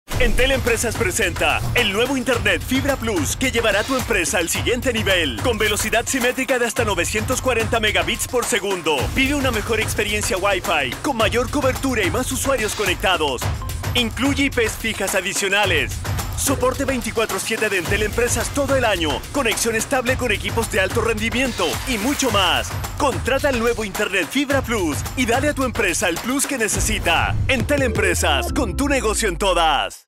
Más conectividad para tu empresa con Internet Fibra Plus
Dale el plus que tu empresa necesita con Internet Fibra Plus. Más usuarios conectados, más cobertura, IP’s fijas y mucho más.